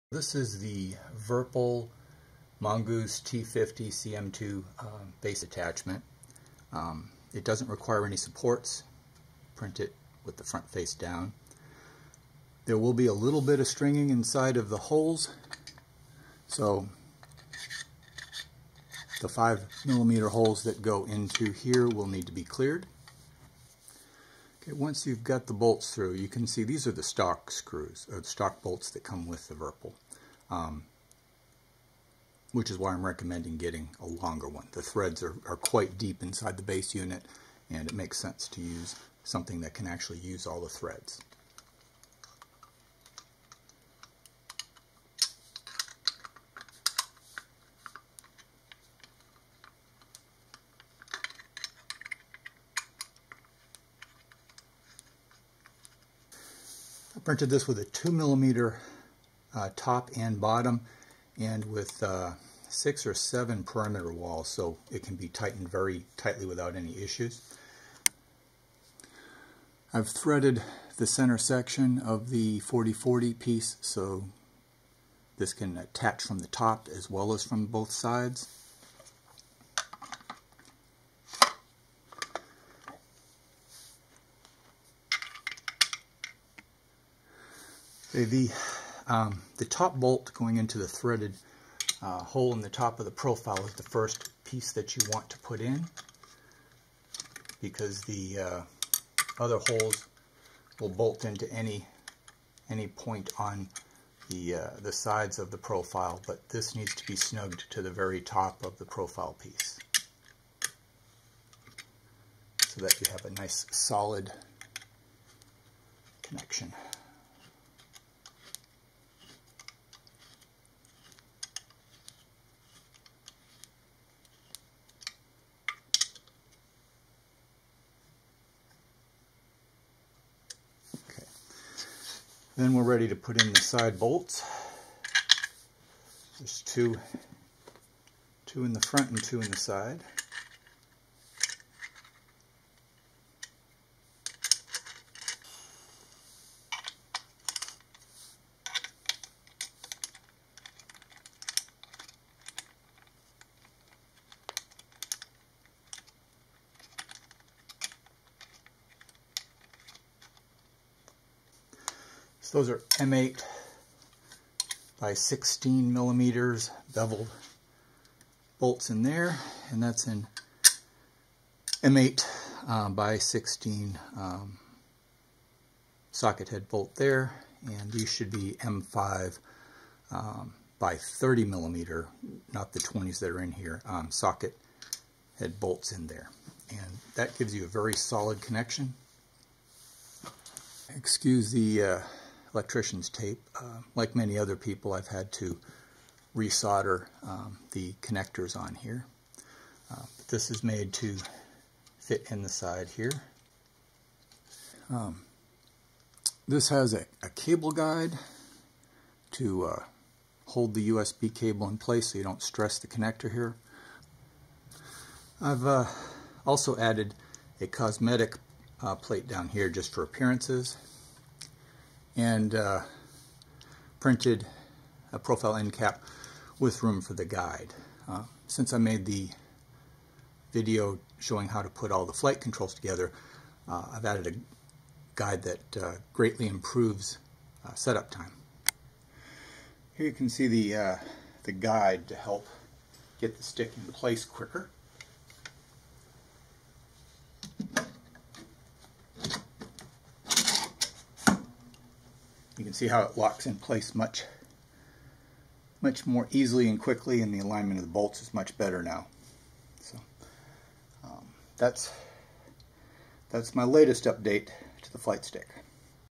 Flight Stick Mount Upgrade
I designed a 3D printed Virpil MongoosT-50 CM2 mount that attaches to the top of a piece of 40x40 profile.  This shows how it mounts, the clearance for the 5pin din connector, cable guide and the alignment guide that I added to help get the stick in place faster.